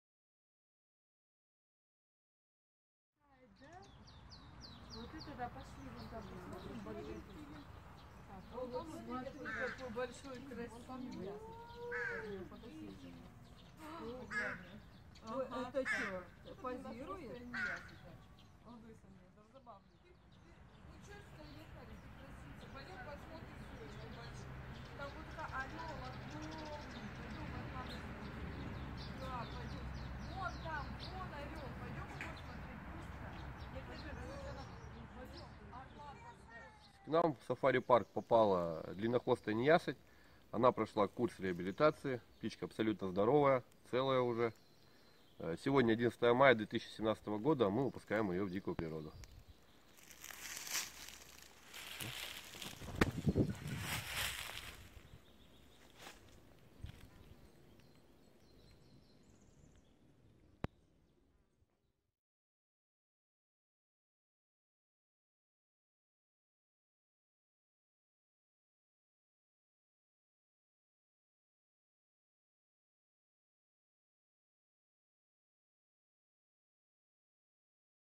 Выпуск в природу длиннохвостой неясыти 11 мая 2017 г.
Загружаем на ютуб видеоролик "Выпуск в природу длиннохвостой неясыти 11 мая 2017 г."

Дмитрий Мезенцев      директор Приморского Сафари-парка